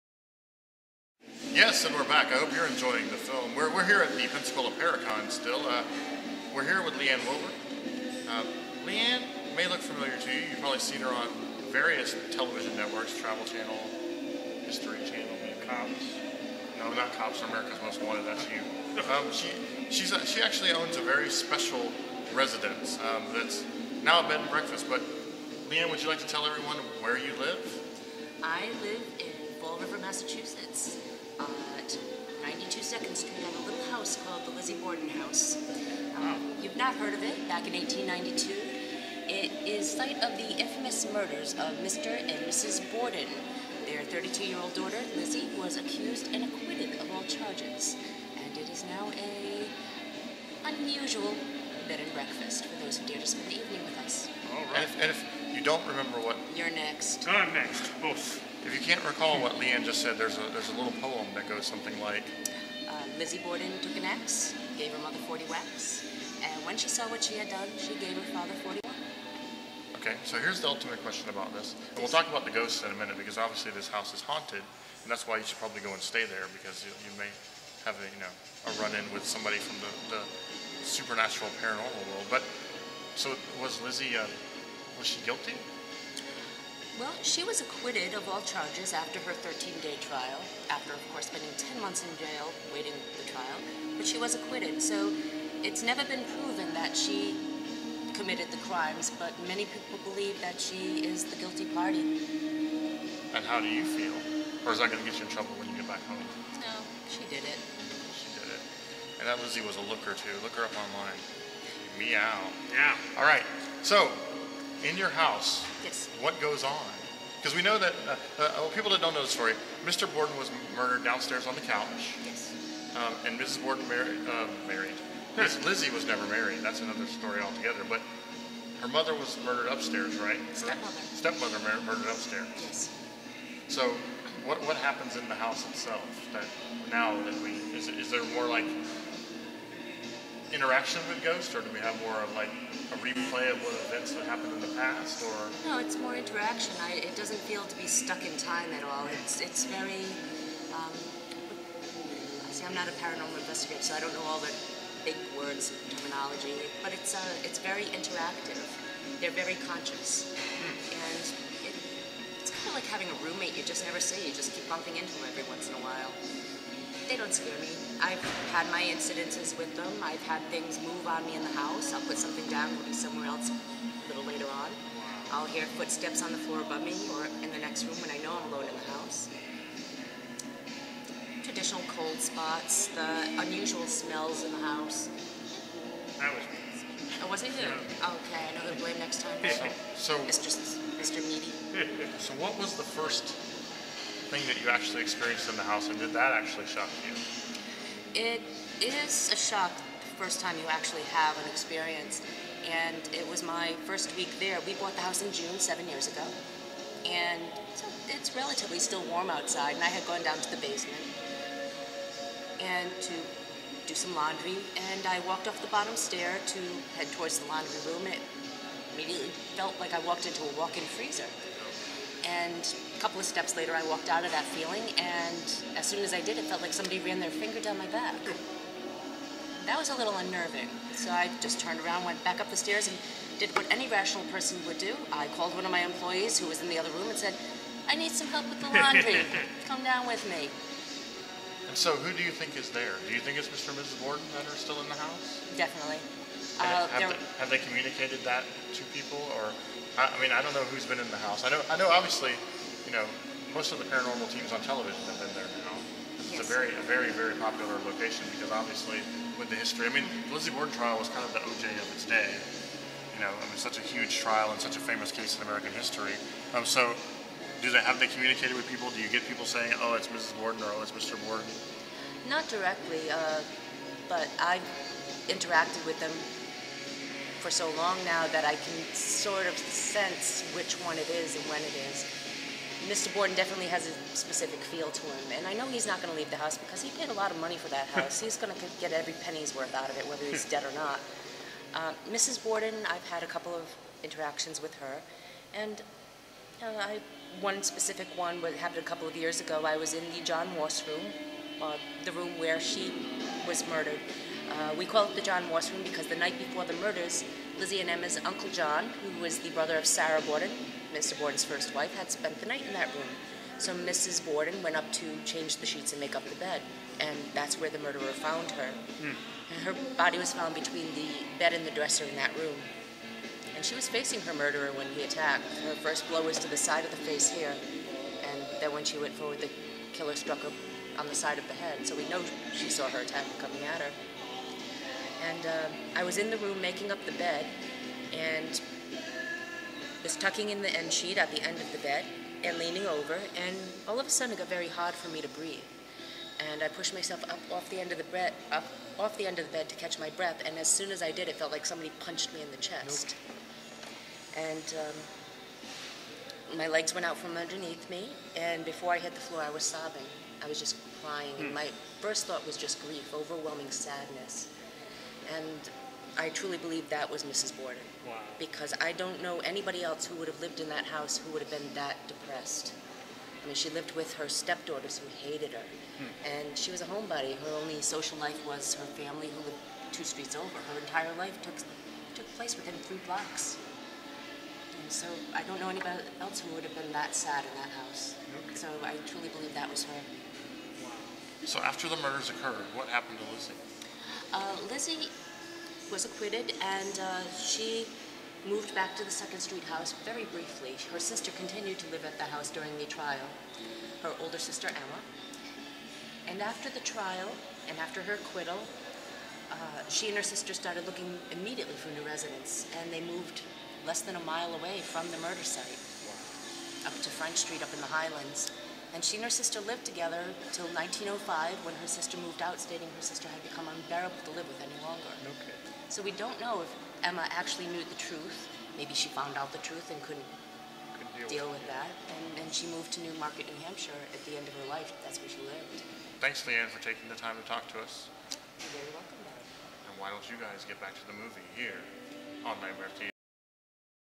Nightmare Theatre at Paracon 2011 - Lizzie Borden Bed and Breakfast
Nightmare Theatre invades the first annual PARACON Paranomal Convention in Pensacola Florida! For more info on Nightmare Theatre, check out